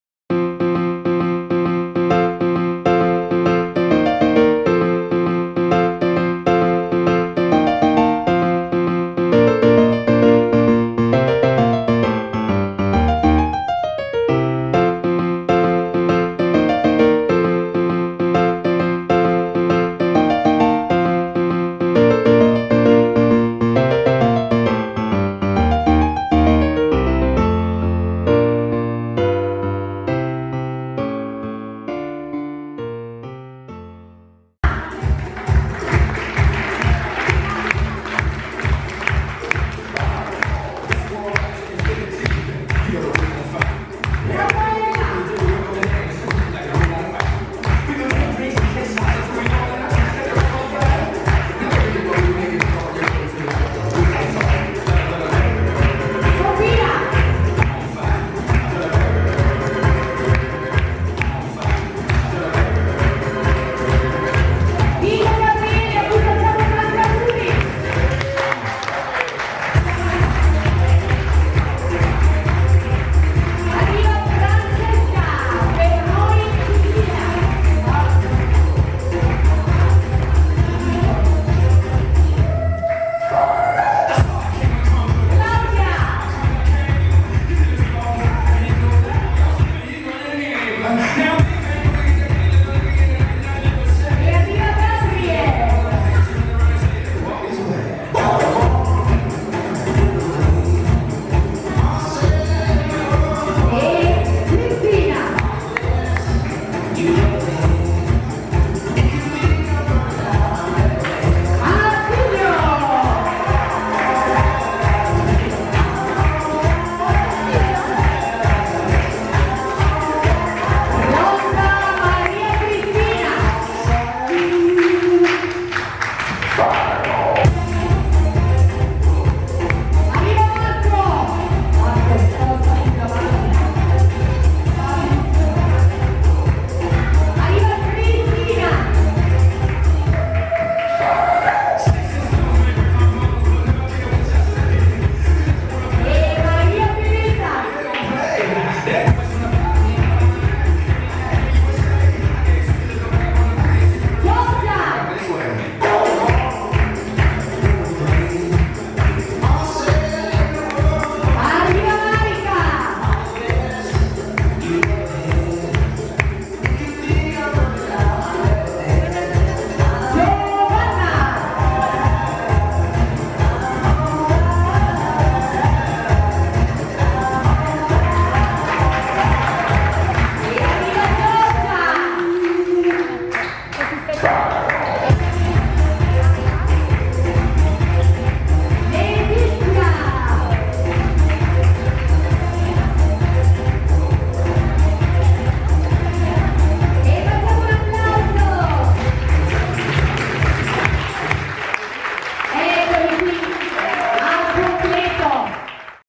Joy Fit - Ragazzo In Forma 2019
Putignano, sabato 15 giugno 2019, Palestra Joy Fit. Saggio di fine anno del gruppo "Ragazzo In Forma" della maestra Agata Marchitelli.